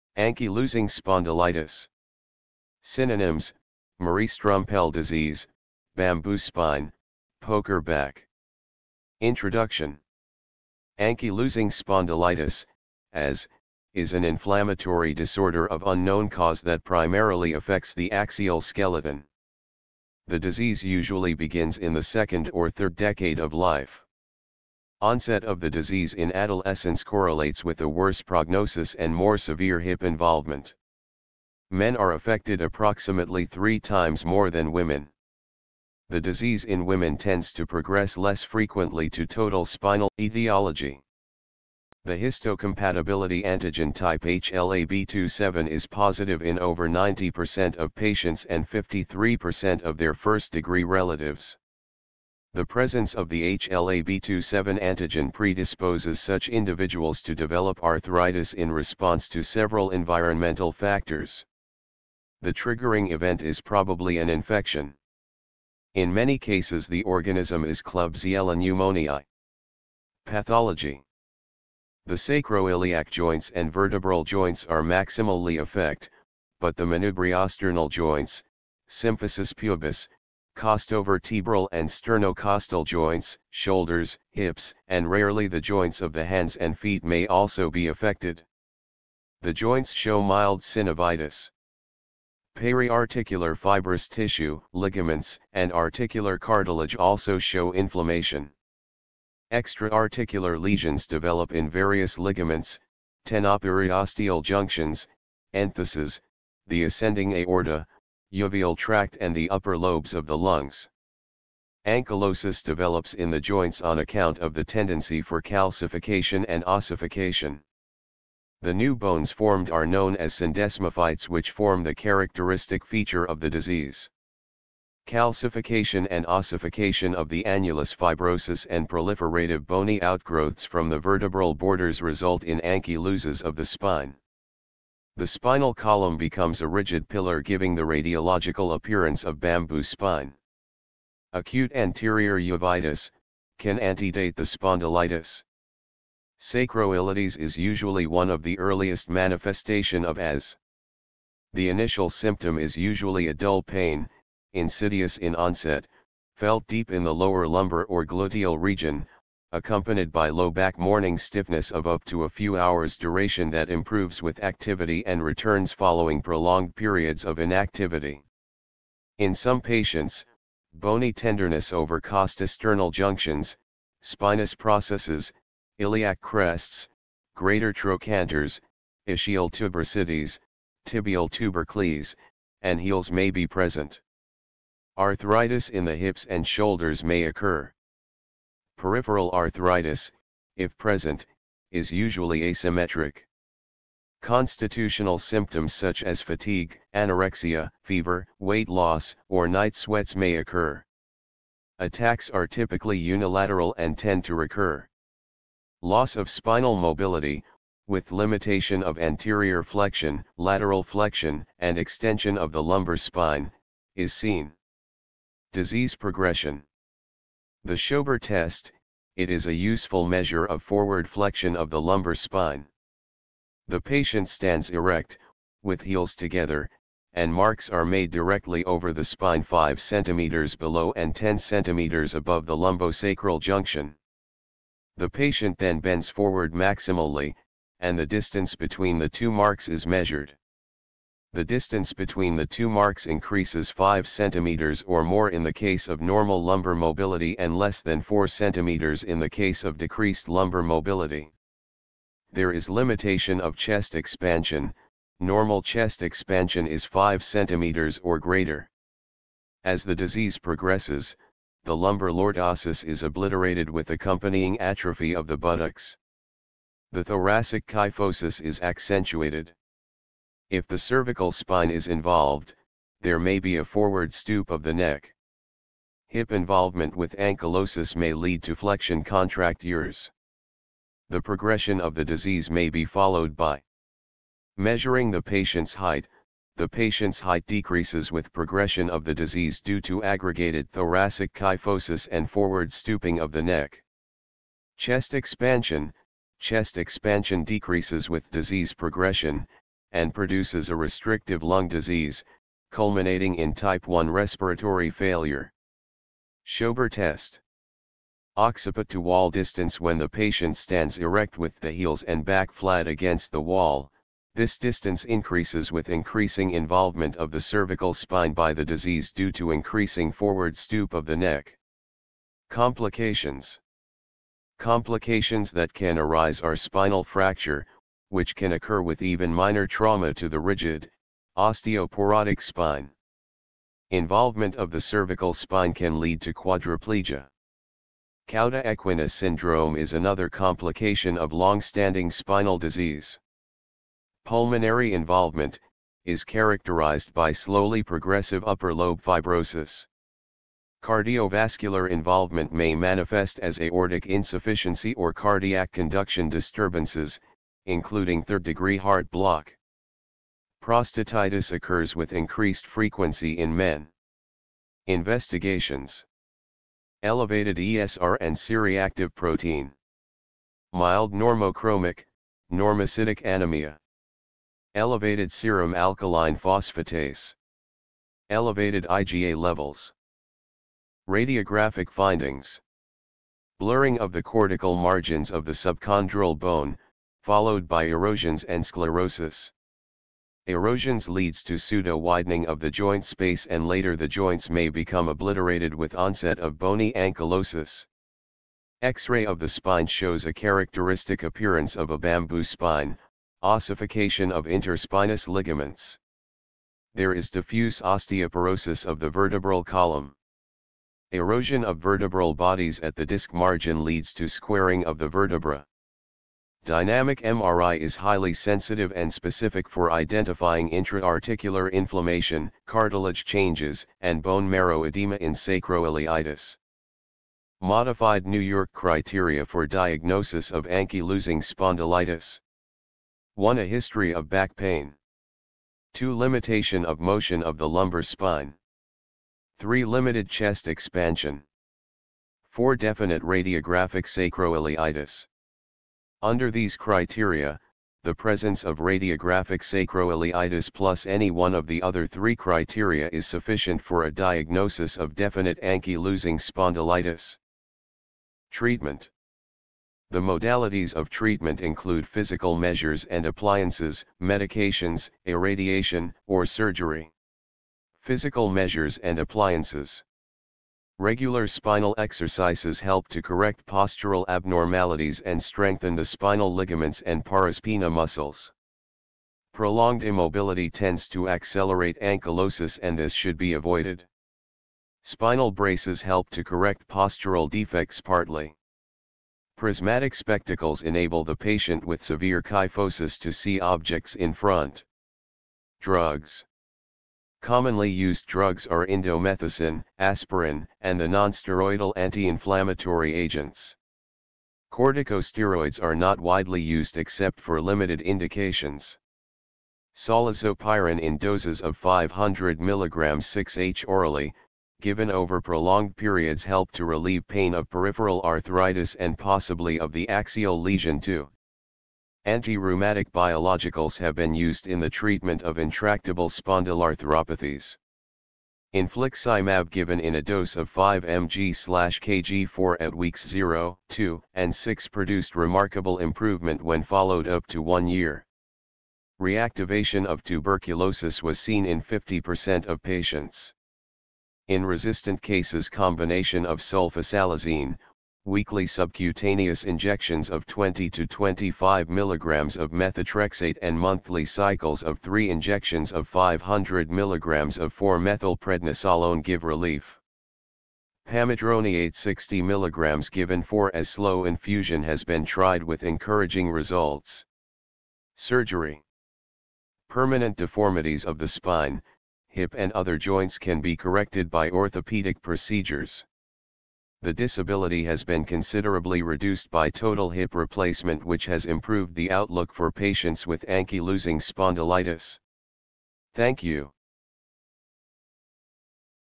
Ankylosing spondylitis/ bamboo spine/ poker back: causes, pathology, symptoms, diagnosis, treatment.
Ankylosing spondylitis (Also known as Marie-Strümpell disease, Bamboo spine, Poker back) general aspects including introduction, etiology, pathology, clinical features, investigation, and treatment.

You may also like presentation on